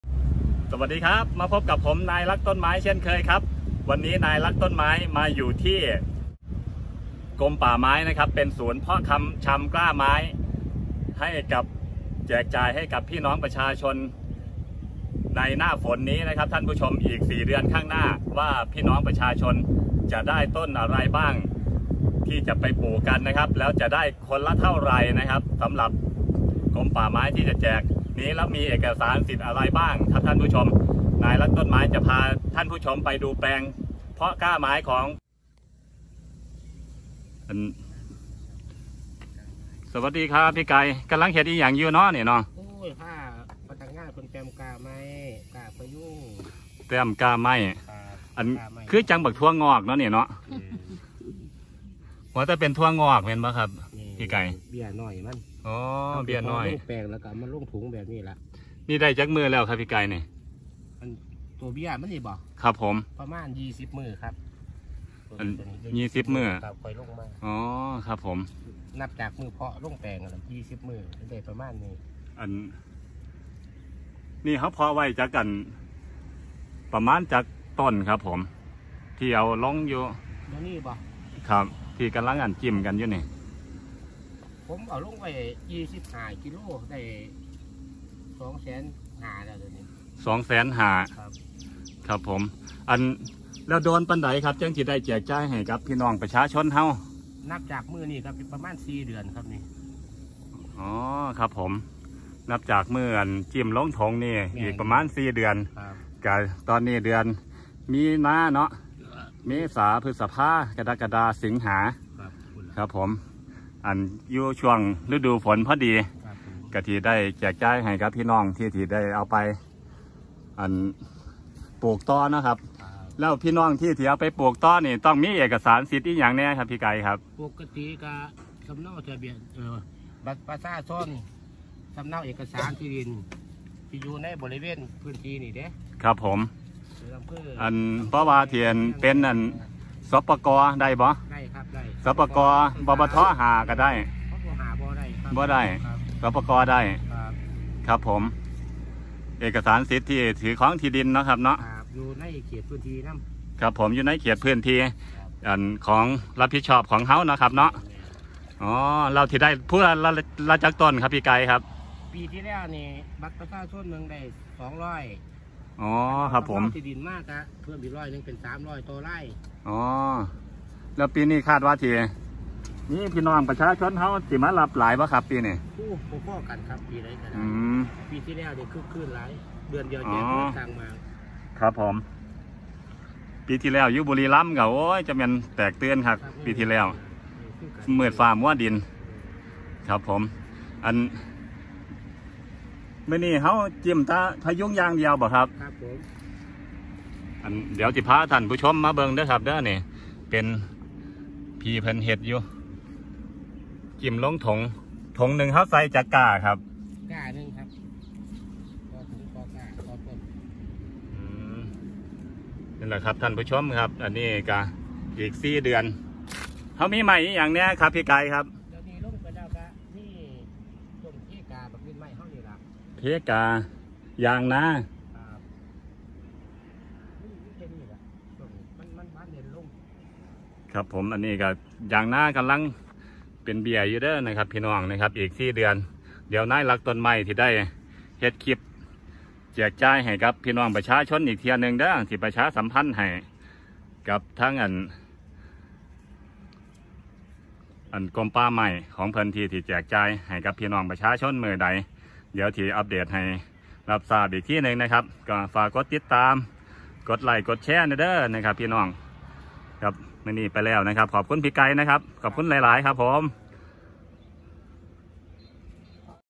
กรมป่าไม้-เตรียมแจกกล้าไม้ฟรีในปี63-อีก4เดือนข้างหน้า EP.1
นายรักต้นไม้ กรมป่าไม้-เตรียมแจกกล้าไม้ฟรีในปี63-อีก4เดือนข้างหน้า